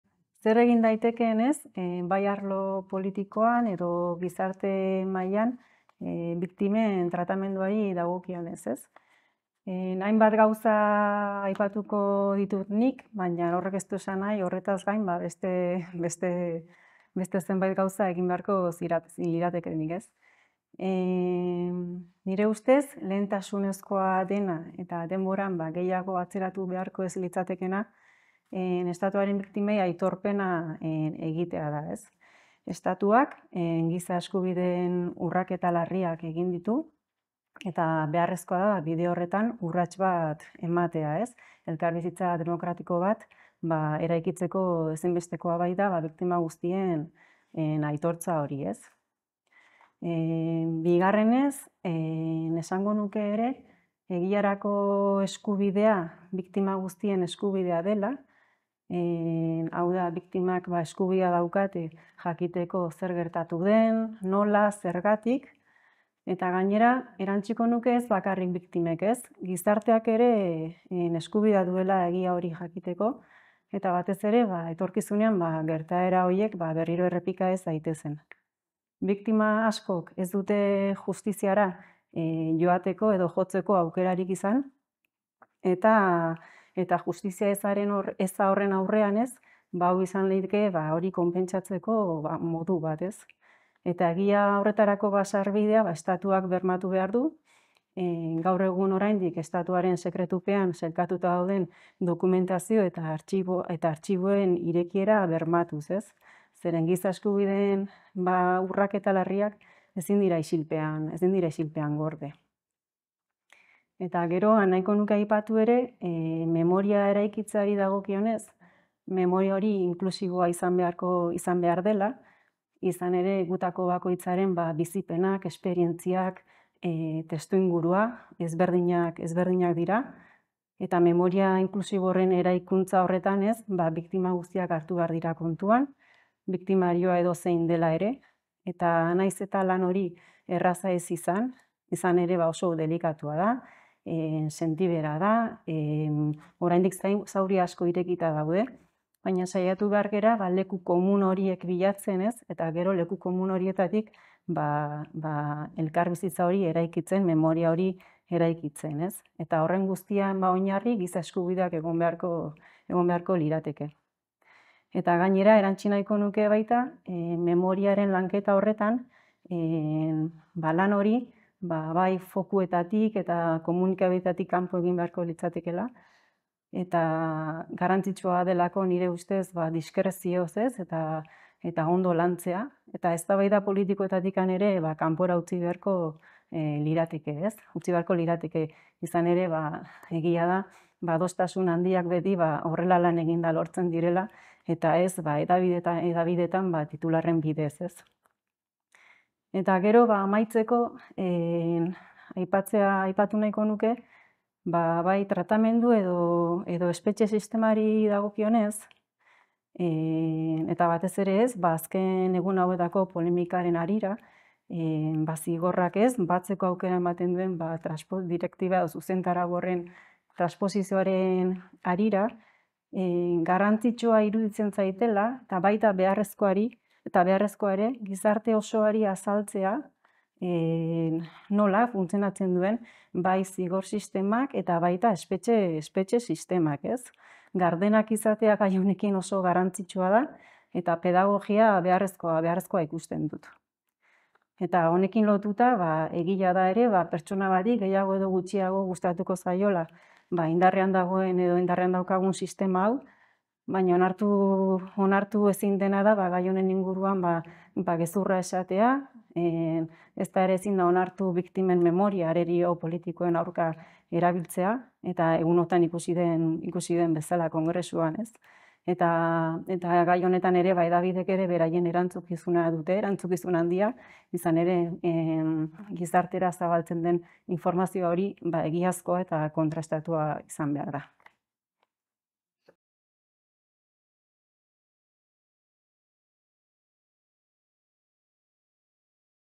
U26 Konferentzia | Maria Jauregi - ETAren Biktima
Maria Jauregi -  ETAren Biktima

Zer egin daitekeen, bai arlo politikoan edo gizarte mailan. Biktimen tratamenduei dagokionez?

Urriaren 26 SAREk antolatutako Konferentzian parte hartu dugu mezu honen bidez